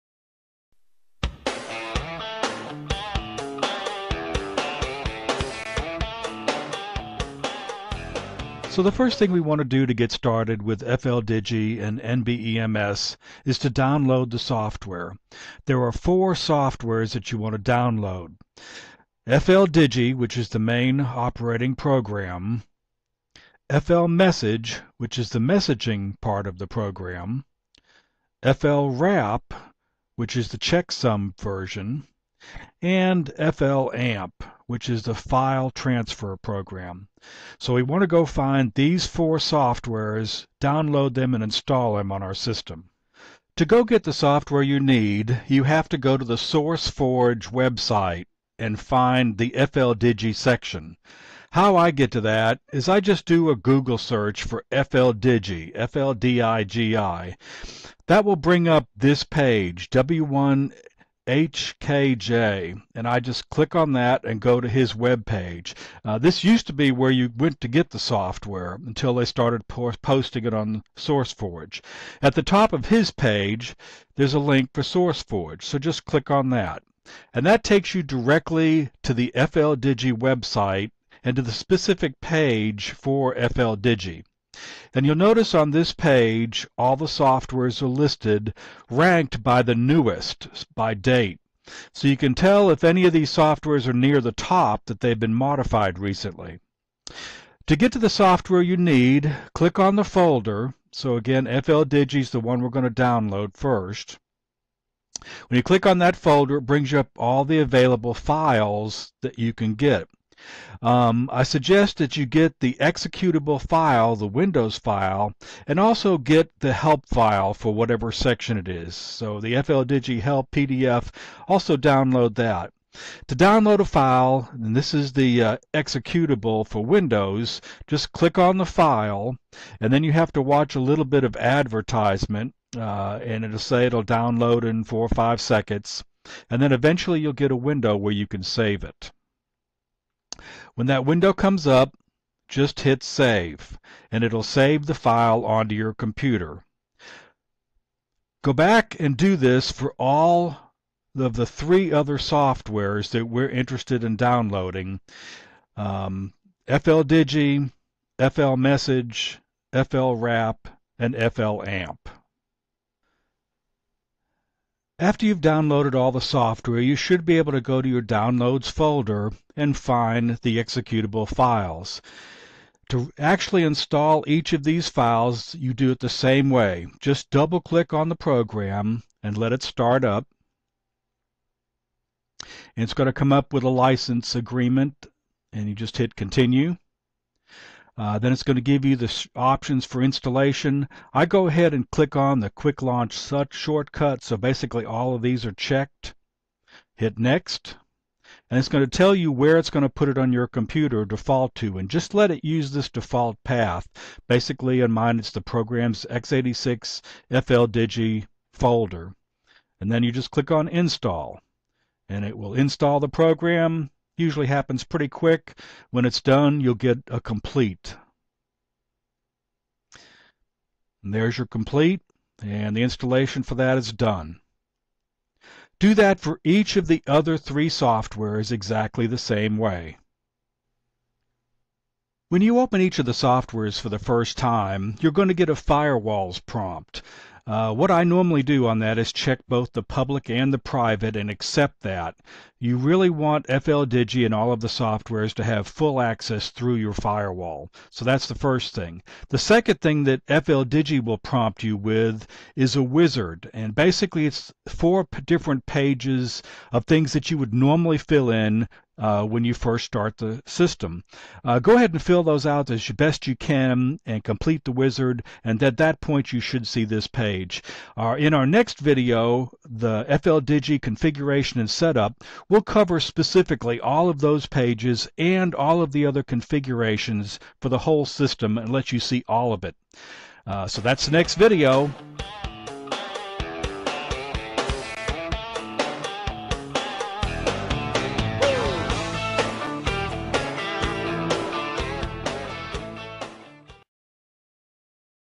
1-"Getting Started with Fldigi/NBEMS"
First in a series on learning to use Fldigi and NBEMS.